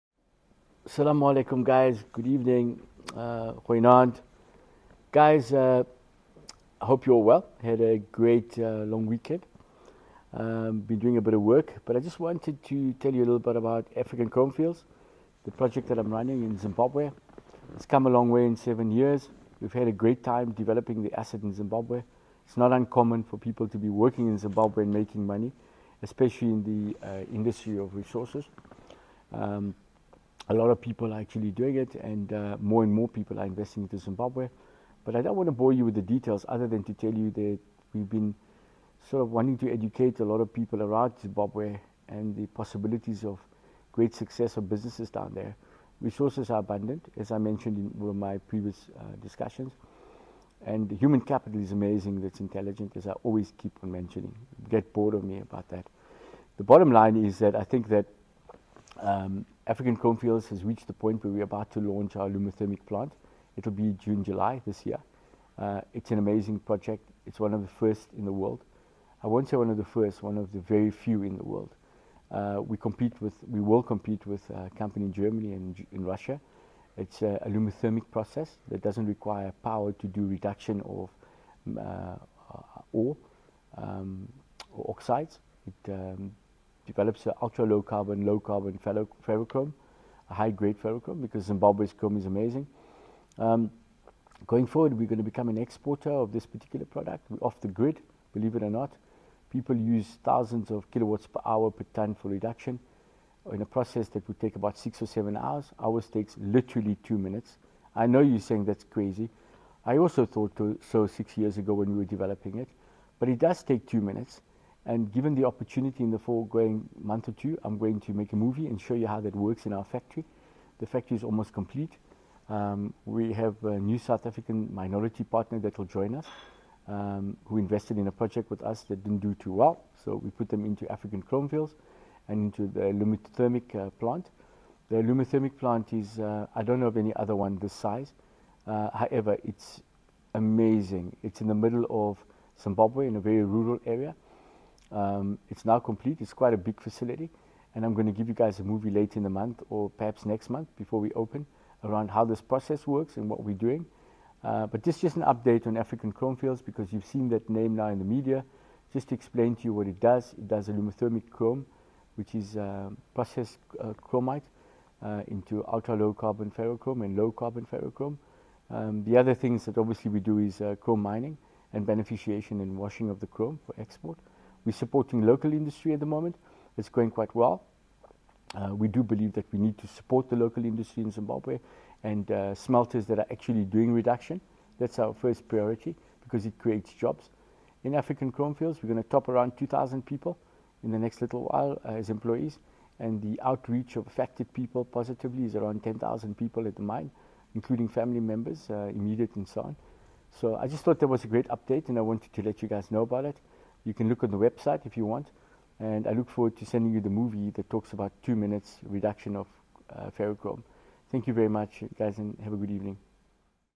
Zunaid Moti to unveil African Chrome Fields ground-breaking smelting factory in Zimbabwe
African Chrome Fields to unveil ground-breaking smelting factory in Zimbabwe

Zimbabwe's mining industry is set to receive a significant boost as a pioneering new chrome smelting factory nears completion, which will place the country firmly on the map in mining circles. The company behind the ground-breaking facility is set to revolutionise the sector with its cutting-edge, proprietary aluminothermic technology and commitment to sustainability, offering a beacon of hope for a region that has long suffered from economic instability.

This company is African Chrome Fields, which has injected some $250 million in investments into the underdeveloped Midlands province since the launch of its operations in 2014. A privately-owned mining company, it has since become the largest chrome miner in Zimbabwe, which is home the second largest chrome reserves in the world.

Its new aluminothermic smelting plant cost some $40 million alone, and in addition to stimulating job creation, will position Zimbabwe as a global frontrunner in ferrochrome production – an ingredient essential for stainless steel manufacturing.

Notably, the new factory’s state-of-the-art technology does not utilise any electricity in the reduction of chrome ore to ferrochrome, a key advantage in an area with little power infrastructure. Additionally, by avoiding chemical contaminants, the process is substantially “greener” and more ecologically friendly than other beneficiation processes.

“As the result of our continued commitment to innovation, the new plant is truly a game-changer for chrome beneficiation,” says Zunaid Moti, Managing Director of African Chrome Fields.

“Our proprietary aluminothermic processes produces ultra-low carbon, high-grade ferrochrome in just a fraction of the time required by traditional methods. This means that we will soon be producing a superior product than can be found nearly anywhere else in the world.”

Moti explains that the technology is so game-changing that African Chrome Fields has refrained from publishing information that would reveal any trade secrets in an attempt to safeguard its intellectual property and ability to establish a copyright, patent or proprietary protection.

“It would have been improper form to fully disclose the actual processes and ingredients used, which may have allowed others to unfairly consider the information and possibly attempt to replicate it.”

The plant marks the latest milestone in the company’s expansion strategy, underscoring its commitment to environmental sustainability and economic growth. While it will initially only operate at 40% capacity before gradually scaling up its output, it will support African Chrome Fields’ mineral beneficiation activities, benefiting its extensive list of well-known South African and international clientele.

Zimbabwe has proven an ideal location for African Chrome Fields to call home, as the company’s reserves are mined from an easily accessible alluvial deposit along the land’s surface. Additionally, the quality of its chrome ore is of a higher grade than many of its international counterparts.

Over the past few years, African Chrome Fields has vastly expanded its operations to include seven chrome processing plants and 19 modular wash plants, with a total template capacity of approximately 1,000 tonnes per hour. The company has also invested around R200 million in mineral exploration over the past six years.

Despite delays due to the pandemic and regional challenges, the construction of the aluminothermic plant, which began in 2016, is now reaching completion. The plant will initially produce limited tonnage of low and ultra-low chromium products, but Moti is confident about the plant's potential.

"Zimbabwe and African Chrome Fields now have the opportunity to compete with major players like Elektrowerk Weisweiler (EWW) in Germany and a factory in Russia that effectively control the global market,” he says.

Ferrochrome, a blend of iron and chrome, is a vital ingredient in stainless steel production. With the increasing demand for stainless steel due to its longevity and resistance to oxidisation, African Chrome Fields plans to gradually increase production to meet rising demand.

Moti is optimistic about the future of the mining industry in Zimbabwe. "Mining is essential to the future of Zimbabwe as a major economic contributor. By helping the country localise its value-added and beneficiation activities, we hope to ensure that surrounding communities benefit from these opportunities rather than seeing the profits funnelled overseas.

The new plant is a testament to African Chrome Fields’ commitment to sustainable mining practices and economic development in the region. "As an African business, we are proud to be expanding our operations in the area. We are committed to supporting its economy and developing supply chains in this unique region," concludes Moti.